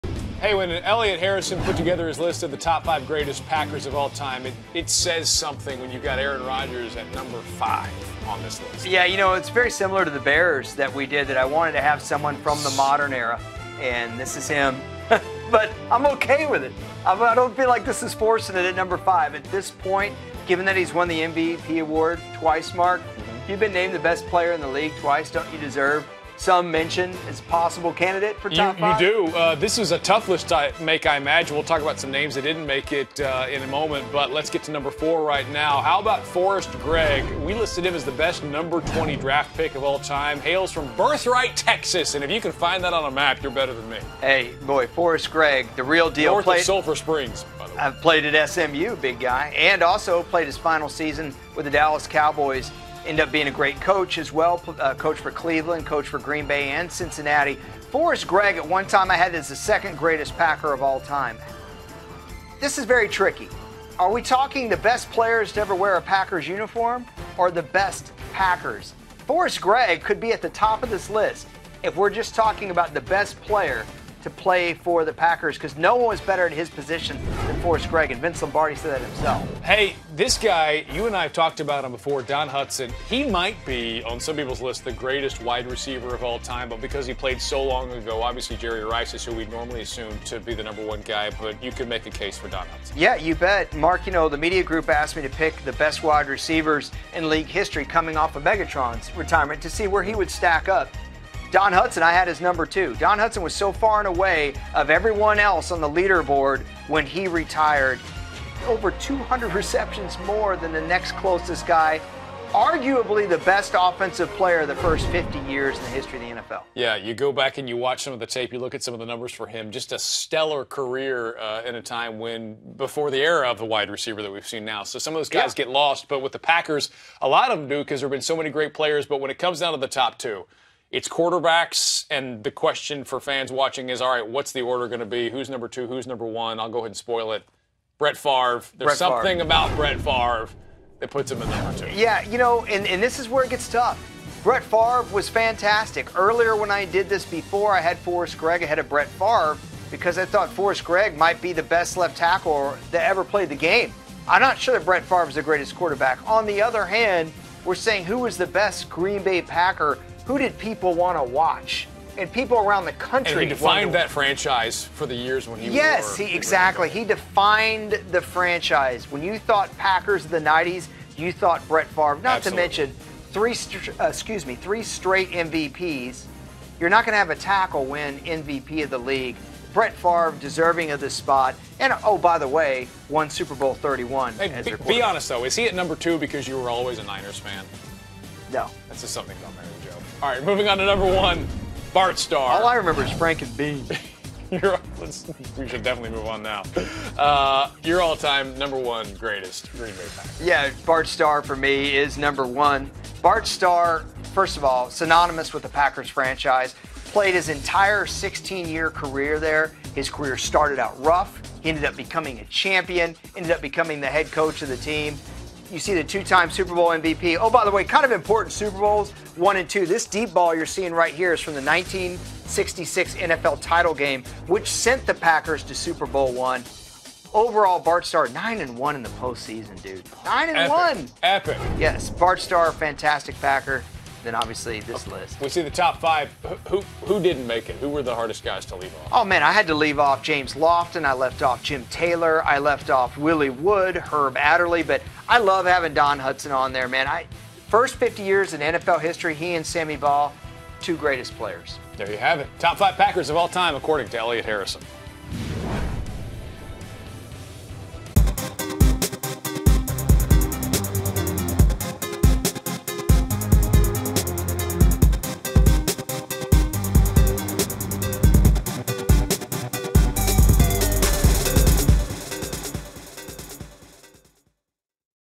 Top 5 Packers of All Time | NFL
Elliot Harrison reveals his list of the Top 5 players in the history of the Green Bay Packers.

1. Bart Starr (QB)
2. Brett Favre (QB)
3. Don Hutson (WR)
4. Forrest Gregg (OT)
5. Aaron Rodgers (QB)